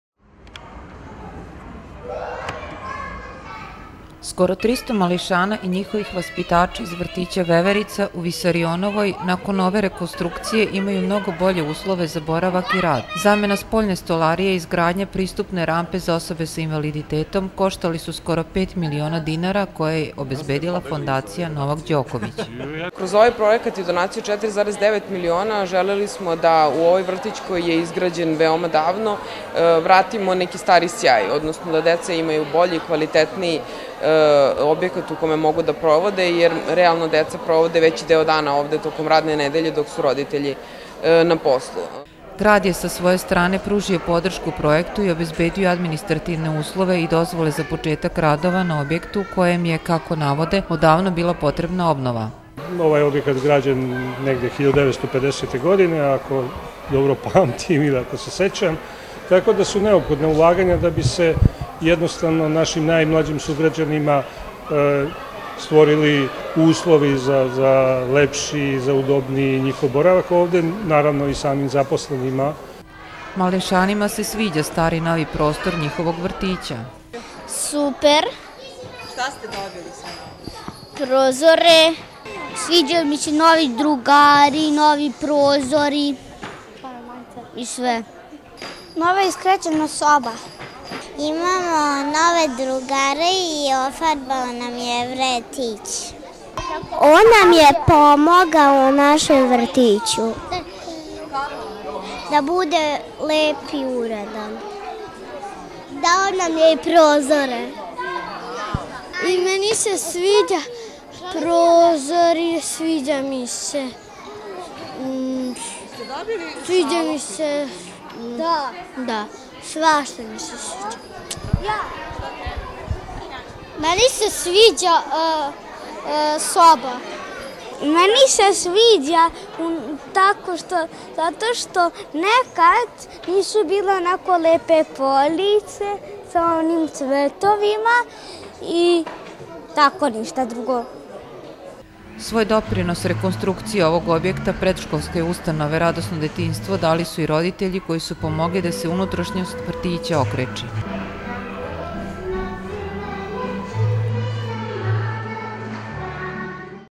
TV KANAL 9, NOVI SAD: OBNOVLJEN VRTIĆ ''VEVERICA''
Danas je svečano je otvoren obnovljeni vrtić ,,Veverica'' u ulici Episkopa Visariona. Ovaj objekat je deo mreže Predškolske ustanove Radosno detinjstvo i trenutno ga pohađa 270 mališana uzrasta od tri do šest i po godina.